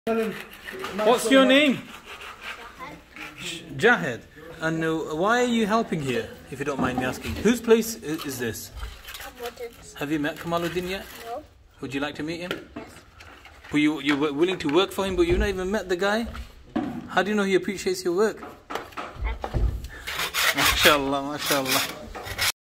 FUNNY Kid doesn't know he's speaking to Kamal Uddin
Young Kid heard of Kamal Uddins New Rawdhatul ilm Education Centre Opening soon and decided to help out not realising he was speaking to the man himself 😂 bless